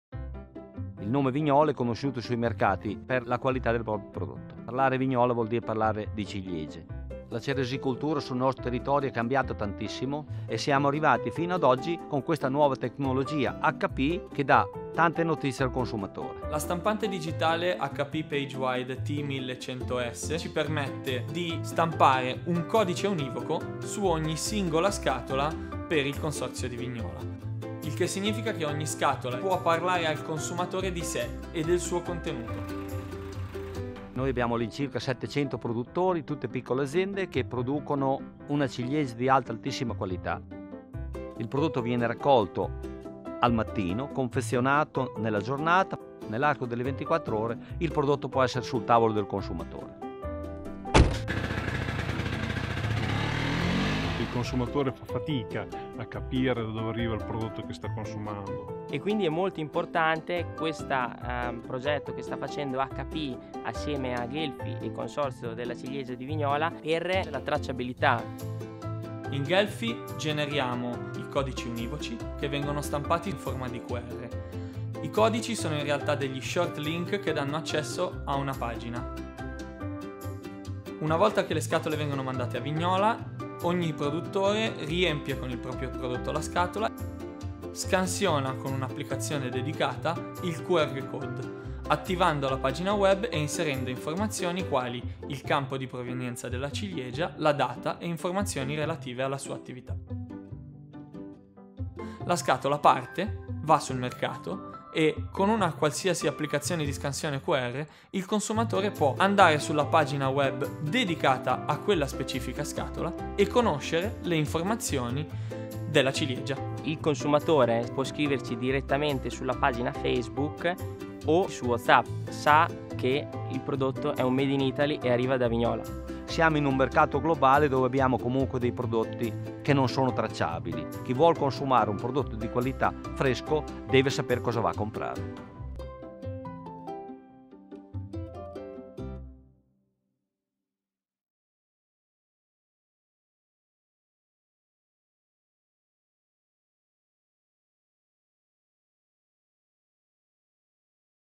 Cherry Boxes Talks to Customers With HP Digital Technology | HP PageWide
Each box of luscious cherries from Italy’s award-winning Vignola Cherry Consortium shares its own unique story with a custom QR code. Designed and printed by Ghelfi Ondulati on the HP PageWide T1100S digital press with food-safe water-based inks, an easy-to-use mobile app traces each corrugated container of perishable fruit from tree to table.

Innovative web- and sheet- digital presses for corrugated packaging, high-volume commercial, and sign and display applications.